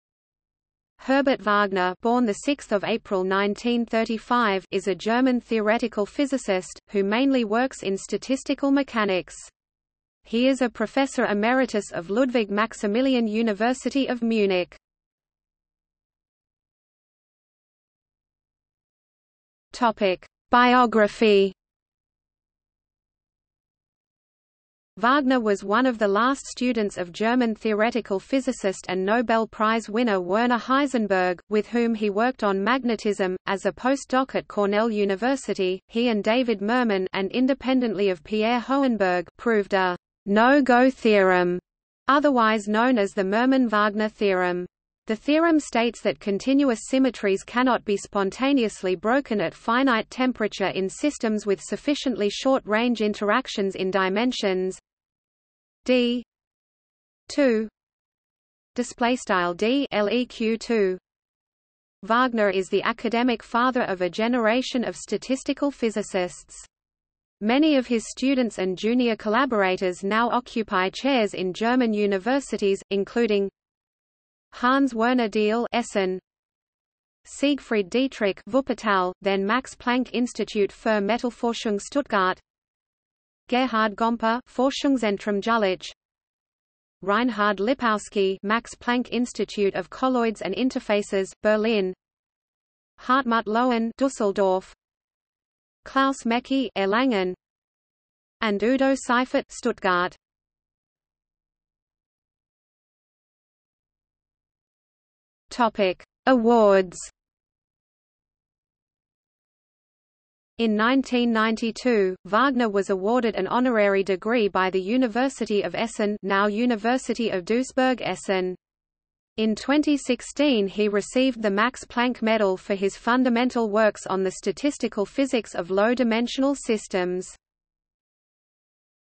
Herbert Wagner (physicist) | Wikipedia audio article
00:00:16 1 Biography
00:01:42 2 Awards

- Socrates

SUMMARY
Herbert Wagner (born 6 April 1935) is a German theoretical physicist, who mainly works in statistical mechanics. He is a professor emeritus of Ludwig Maximilian University of Munich.